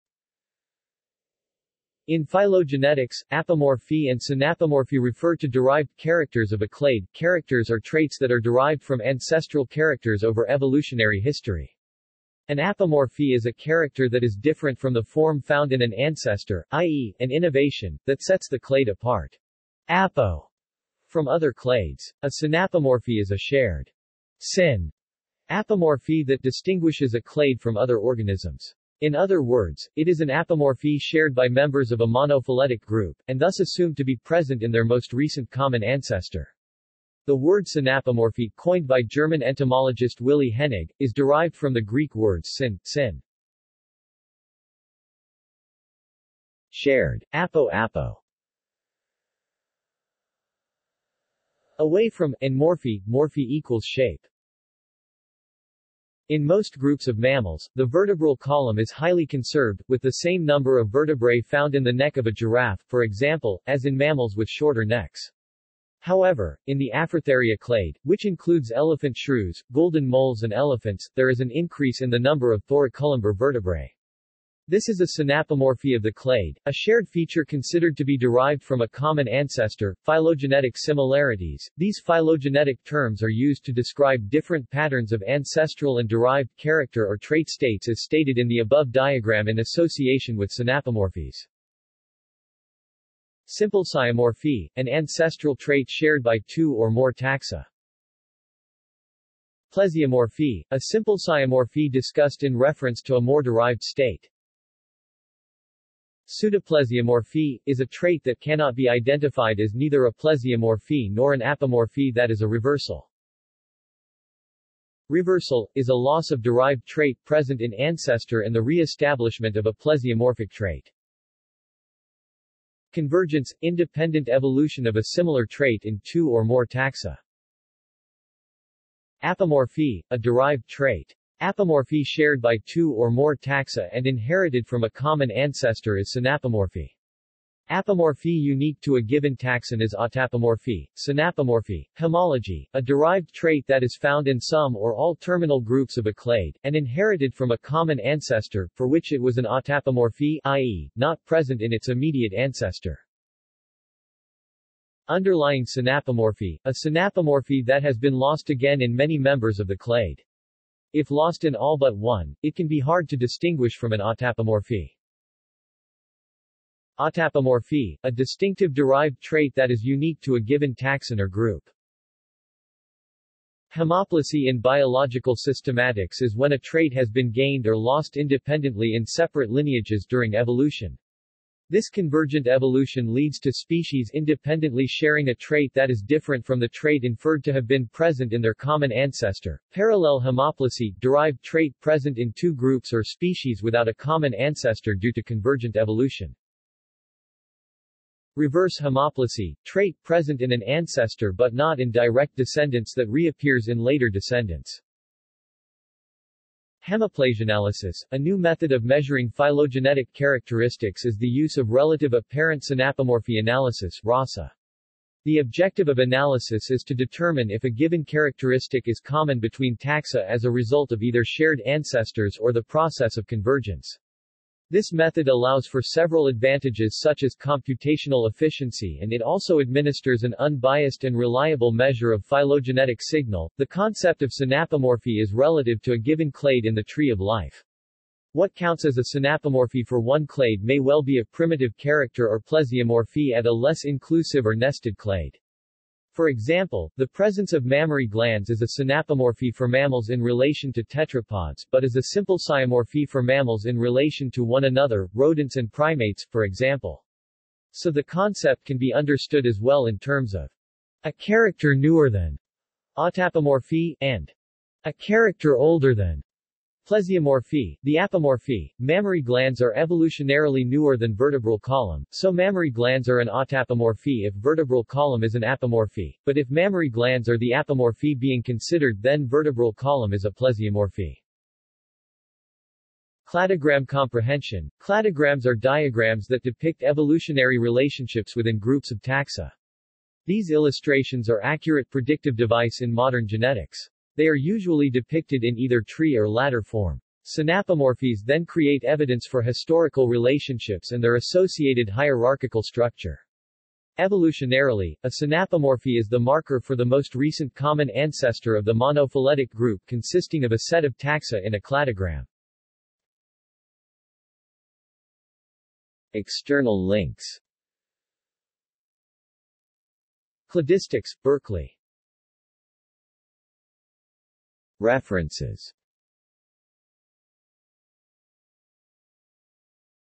Synapomorphy | Wikipedia audio article
This is an audio version of the Wikipedia Article:
Synapomorphy

SUMMARY
In phylogenetics, apomorphy and synapomorphy refer to derived characters of a clade – characters or traits that are derived from ancestral characters over evolutionary history. An apomorphy is a character that is different from the form found in an ancestor, i.e., an innovation, that sets the clade apart ("apo") from other clades. A synapomorphy is a shared ("syn") apomorphy that distinguishes a clade from other organisms. In other words, it is an apomorphy shared by members of a monophyletic group, and thus assumed to be present in their most recent common ancestor. The word synapomorphy, coined by German entomologist Willi Hennig, is derived from the Greek words σύν, syn = shared; ἀπό, apo = away from; and μορφή, morphe = shape.
In most groups of mammals, the vertebral column is highly conserved, with the same number of vertebrae found in the neck of a giraffe, for example, as in mammals with shorter necks. However, in the Afrotheria clade, which includes elephant shrews, golden moles and elephants, there is an increase in the number of thoracolumbar vertebrae. This is a synapomorphy of the clade: a shared feature considered to be derived from a common ancestor.Phylogenetic similarities: These phylogenetic terms are used to describe different patterns of ancestral and derived character or trait states as stated in the above diagram in association with synapomorphies.
Symplesiomorphy – an ancestral trait shared by two or more taxa.
Plesiomorphy – a symplesiomorphy discussed in reference to a more derived state.
Pseudoplesiomorphy – is a trait that cannot be identified as neither a plesiomorphy nor an apomorphy that is a reversal.
Reversal – is a loss of derived trait present in ancestor and the reestablishment of a plesiomorphic trait.
Convergence – independent evolution of a similar trait in two or more taxa.
Apomorphy – a derived trait. Apomorphy shared by two or more taxa and inherited from a common ancestor is synapomorphy. Apomorphy unique to a given taxon is autapomorphy.Synapomorphy/Homology – a derived trait that is found in some or all terminal groups of a clade, and inherited from a common ancestor, for which it was an autapomorphy (i.e., not present in its immediate ancestor).
Underlying synapomorphy – a synapomorphy that has been lost again in many members of the clade. If lost in all but one, it can be hard to distinguish from an autapomorphy.
Autapomorphy – a distinctive derived trait  that is unique to a given taxon or group.
Homoplasy in biological systematics is when a trait has been gained or lost independently in separate lineages during evolution. This convergent evolution leads to species independently sharing a trait that is different from the  trait inferred to have been present in their common ancestor.Parallel Homoplasy –  derived trait present in two groups or species without a common ancestor due to convergent evolution.
Reverse Homoplasy – trait present in an ancestor but not in direct descendants that reappears in later descendants.
HemiplasyAnalysis: A new method of measuring phylogenetic characteristics is the use of Relative Apparent Synapomorphy Analysis (RASA). The objective of analysis is to determine if a given characteristic is common between taxa as a result of either shared ancestors or the process of convergence. This method allows for several advantages such as computational efficiency and it also administers an unbiased and reliable measure of phylogenetic signal.The concept of synapomorphy is relative to a given clade in the tree of life. What counts as a synapomorphy for one clade may well be a primitive character or plesiomorphy at a less inclusive or nested clade. For example, the presence of mammary glands is a synapomorphy for mammals in relation to tetrapods but is a symplesiomorphy for mammals in relation to one another, rodents and primates, for example. So the concept can be understood as well in terms of "a character newer than" (autapomorphy) and "a ch ...